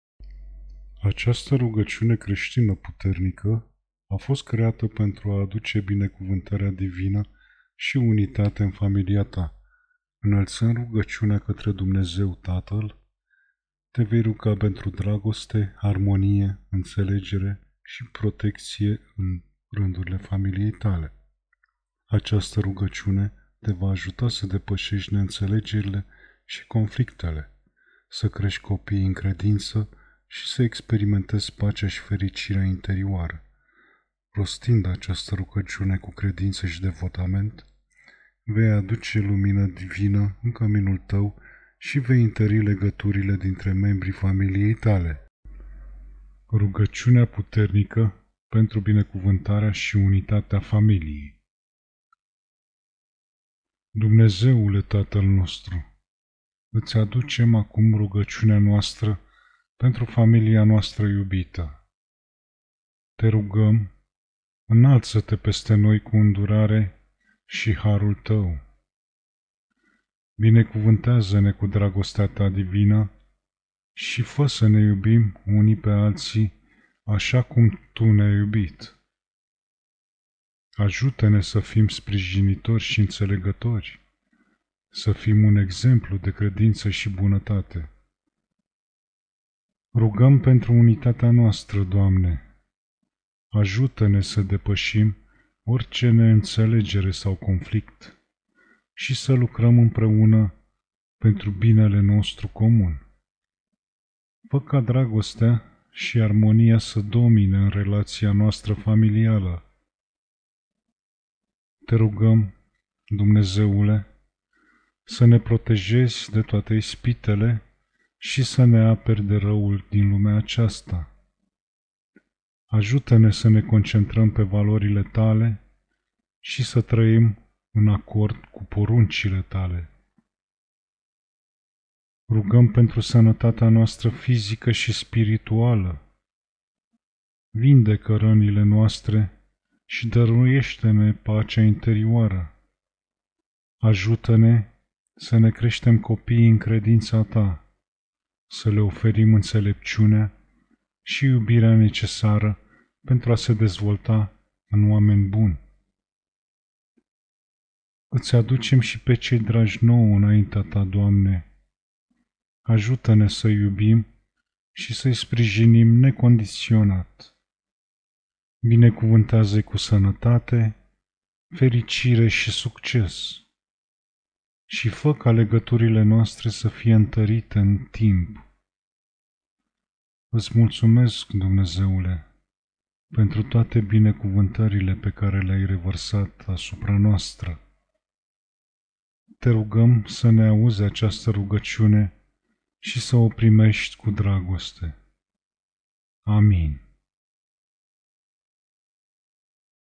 Rugăciunea puternică pentru binecuvântarea și unitatea familiei.
Această rugăciune creștină puternică a fost creată pentru a aduce binecuvântarea divină și unitatea în familia ta. Înălțând rugăciunea către Dumnezeu Tatăl, te vei ruga pentru dragoste, armonie, înțelegere și protecție în rândurile familiei tale. Această rugăciune te va ajuta să depășești neînțelegerile și conflictele, să crești copiii în credință și să experimentezi pacea și fericirea interioară. Rostind această rugăciune cu credință și devotament, vei aduce lumina divină în căminul tău și vei întări legăturile dintre membrii familiei tale.